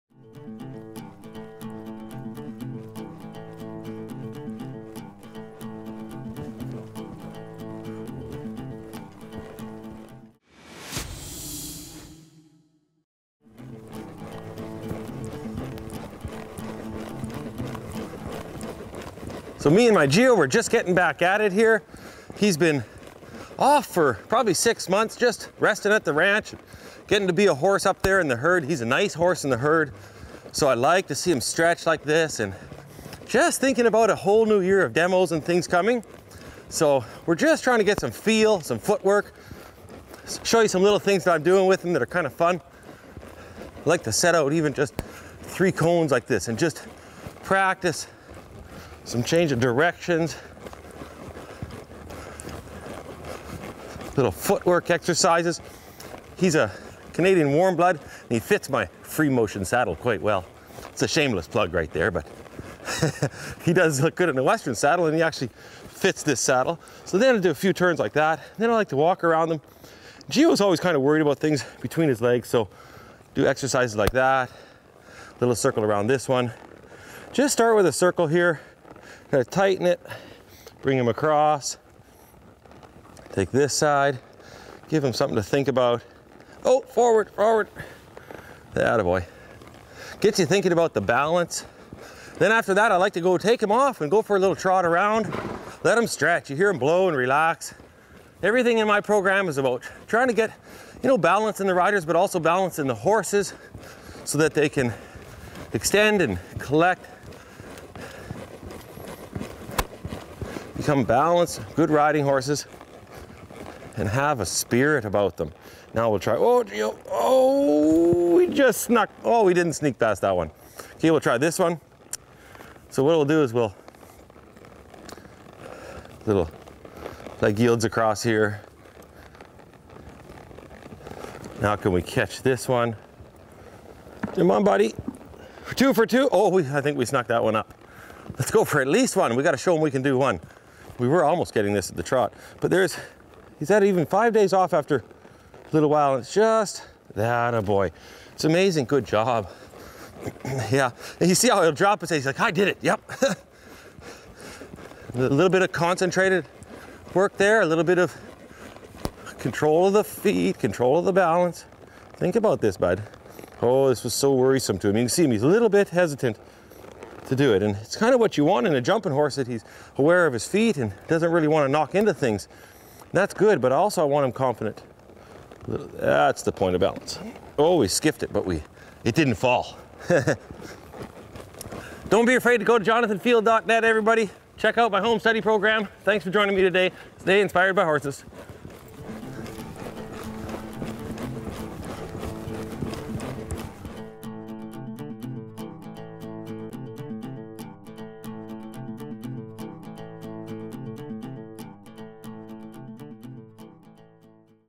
Reconnecting with my Horse after 6 Months Off | Canadian Warmblood Geo & Jonathan Field
In this candid session, join Jonathan Field and his beautiful grey Canadian Warmblood horse Bellagio, affectionately known as Geo, as they come back after a 6-month break. Jonathan is all about teaching and helping others have success with their horses, and in this video, you can get some great ideas to do with your horse at home. Watch as Jonathan focuses on getting feel and footwork and reconnecting with Geo, who is always worried about things around his legs. Learn a simple exercise that you can take home and try with your own horse, and see how it can help Geo become more connected and confident. Don't miss out on this insightful and inspiring session!

Join our free newsletter for updates on Jonathan Field Horsemanship! No spam, no selling of your email, and you can unsubscribe anytime.